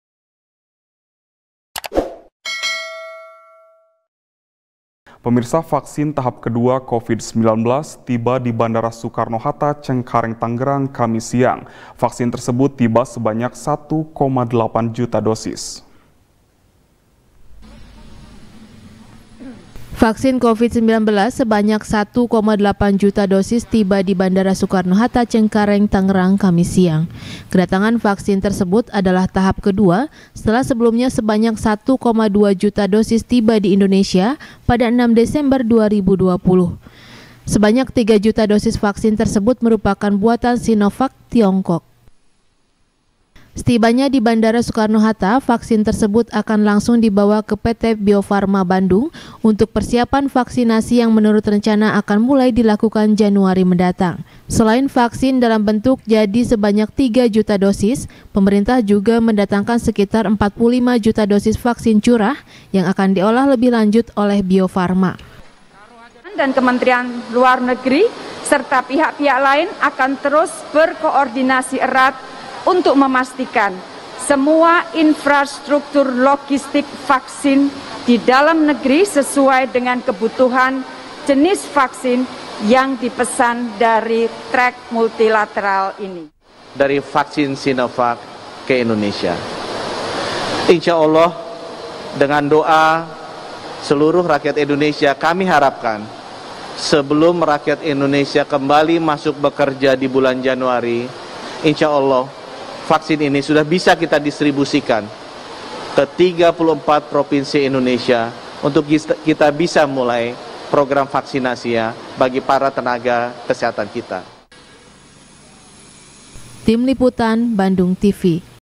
Vaksin Tahap Kedua Tiba Di Indonesia | SBR | BANDUNGTV
SBR | BANDUNGTV | HANY SHANDRA | Vaksin tahap kedua Covid-19 tiba di Bandara Soekarno-Hatta, cengkareng-tangerang, kamis siang. Vaksin tersebut tiba sebanyak 1,8 juta dosis.